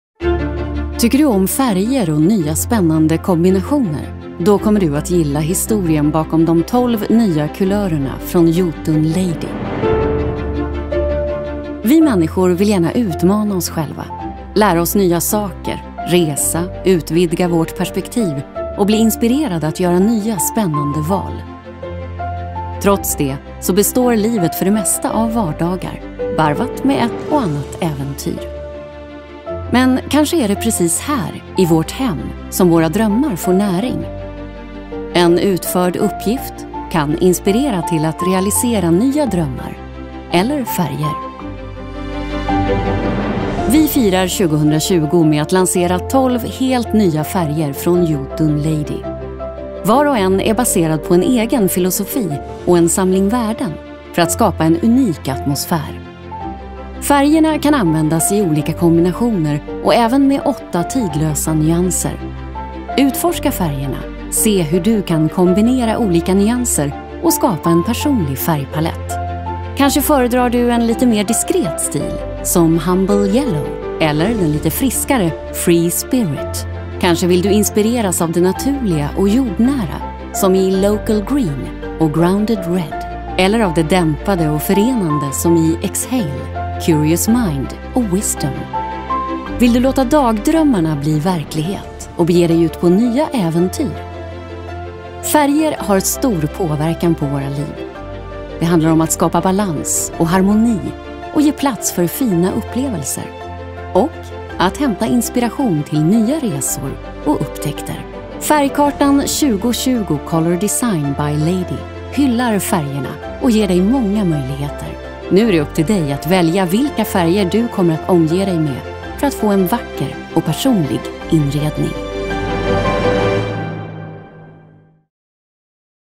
2020 ny färgkarta från Jotun LADY
Jotun LADY 2020 - en hyllning till färgerna. Ny färgkarta från LADY med 12 nya inomhuskulörer som skapar en unik atmosfär i ditt hem. Färgerna kan användas i olika kombinationer och även tillsammans med 8 tidlösa nyanser.